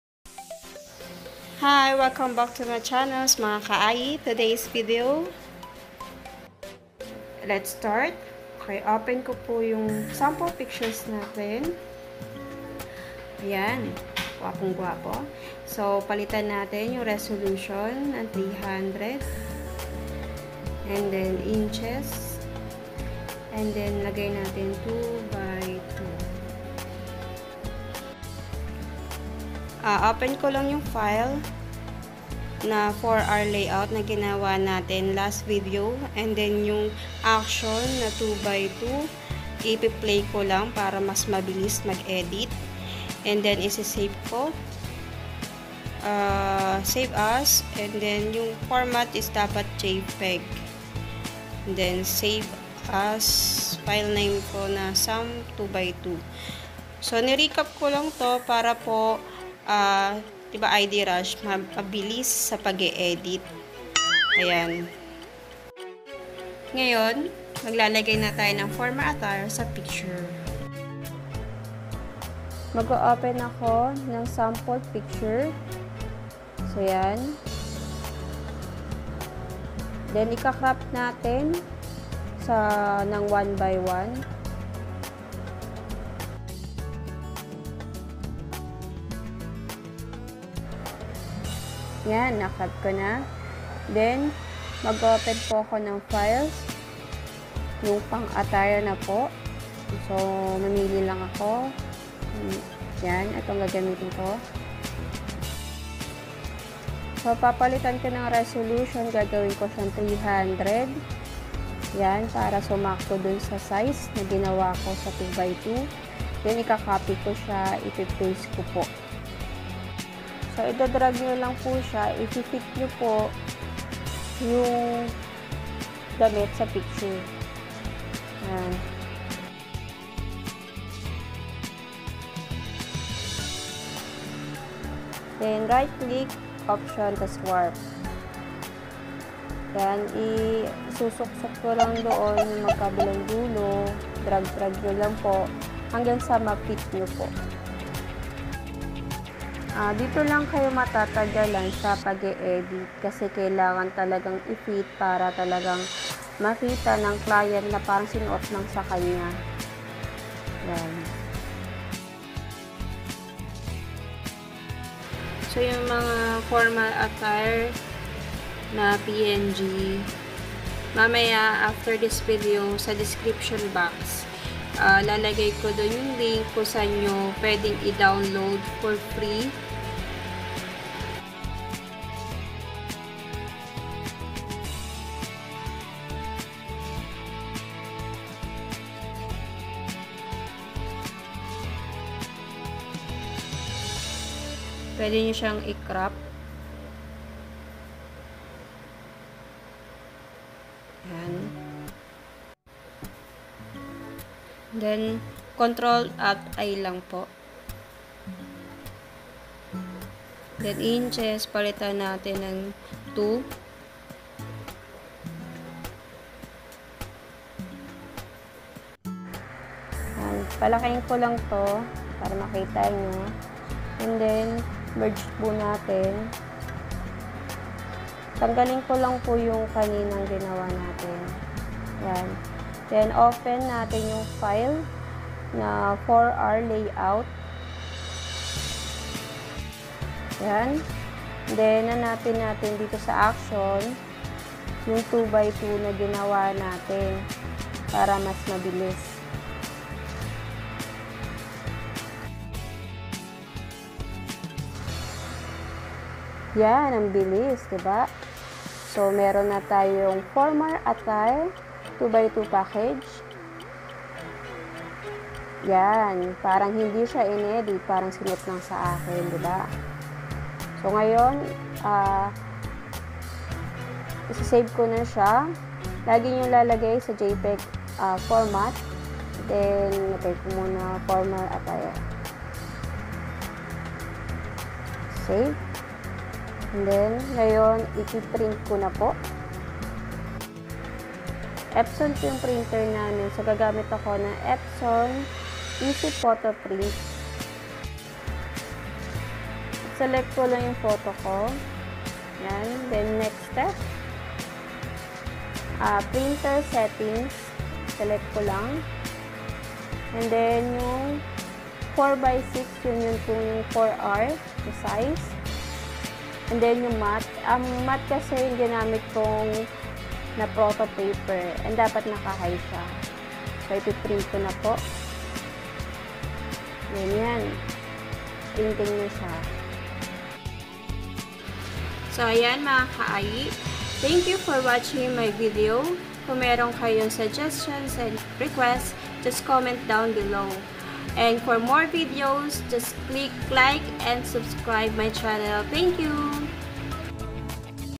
RUSH ID TUTORIAL FORMAL ATTIRE using Photoshop CS6 | TAGALOG VERSION
Para sa beginners palang  sa photoshop, makakatulong video na ito upang mahasa pa iyong kaalaman sa paggamit ng PHOTOSHOP CS6... simple, madali at mabilis mo lang matutunan dito na ito...

link for attire png. LIBRE lang po idownload niyo po.